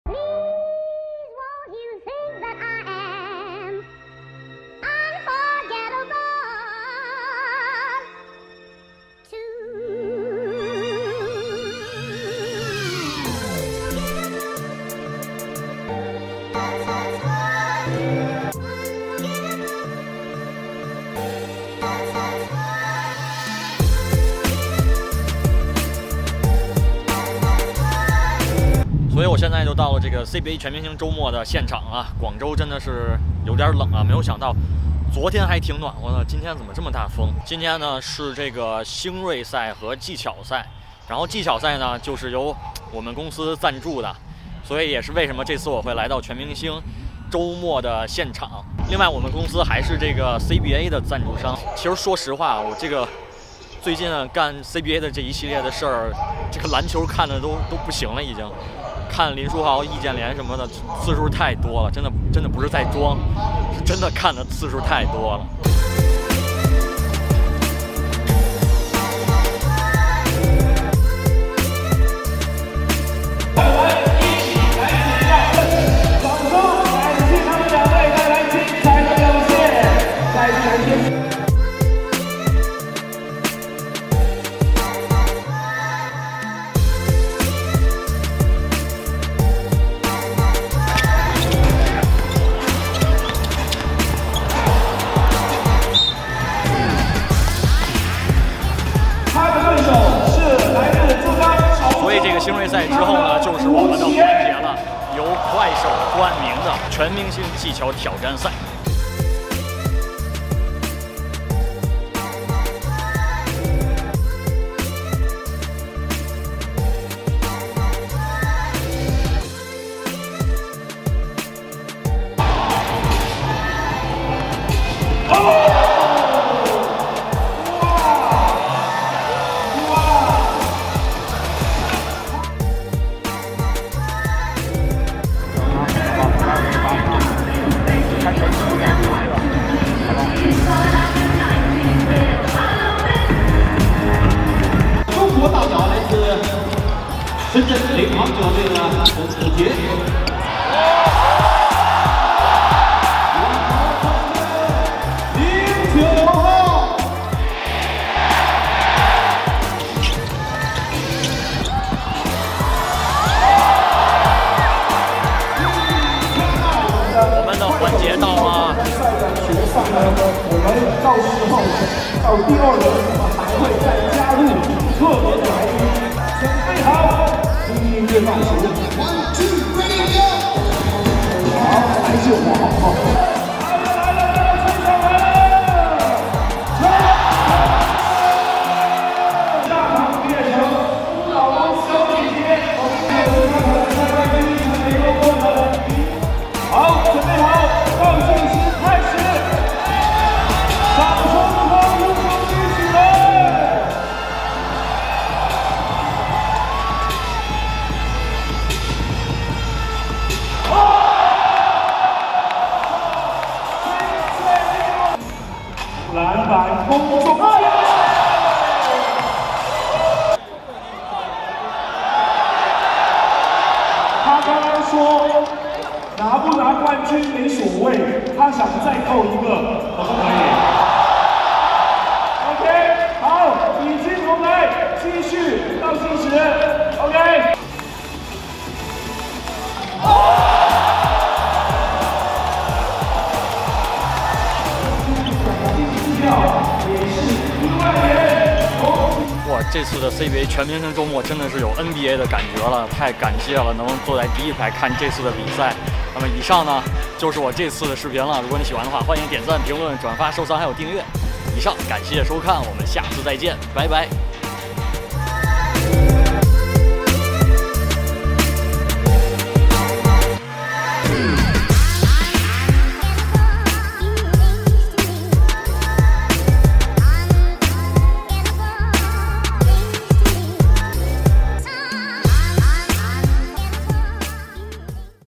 我们赞助了CBA，在第一排看CBA全明星是什么体验，林书豪易建联近在眼前！
微博：滤客_Luc

上一周去广州看了CBA全明星周末，录了一期Vlog分享给大家，确实今年的CBA比往年精彩了很多，越来越有样了！